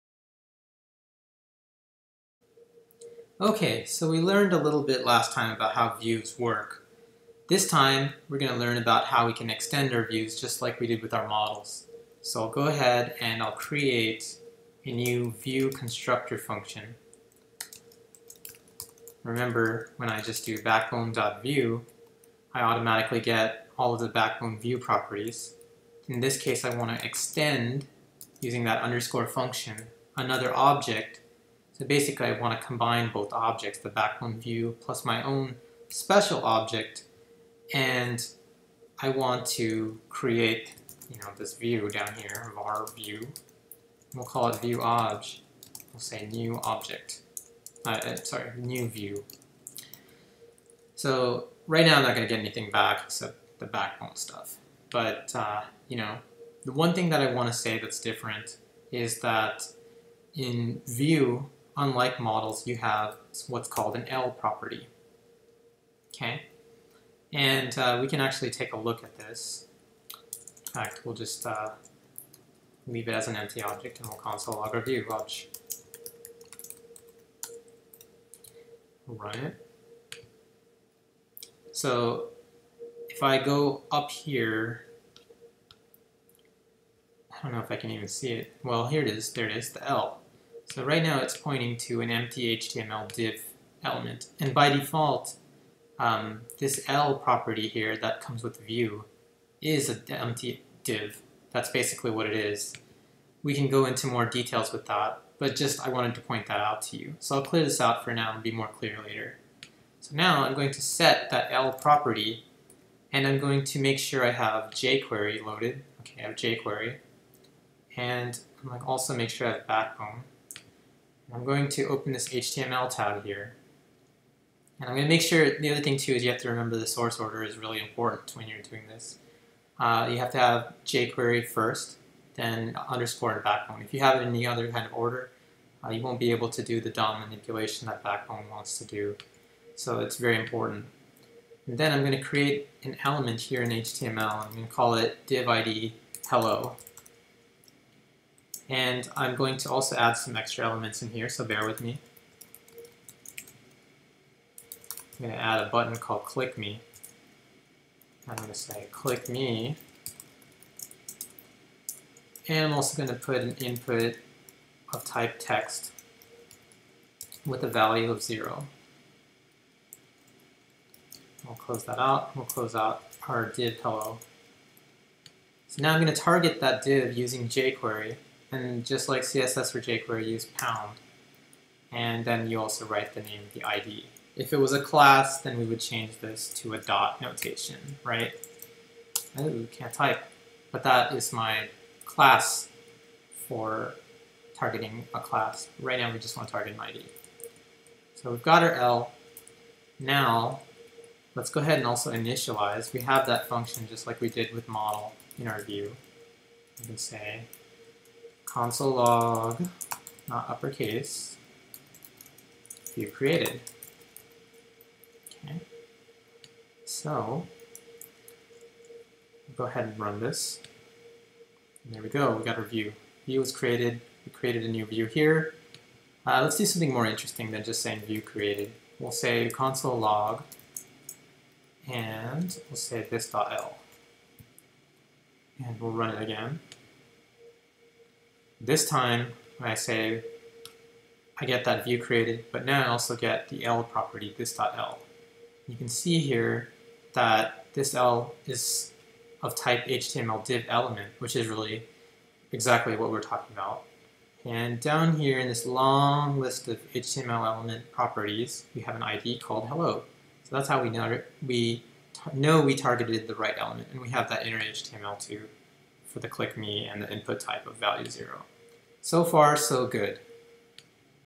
The el Property in Backbone Views
In this video we'll go over the el property of Backbone Views and how it targets elements on the DOM of an HTML webpage.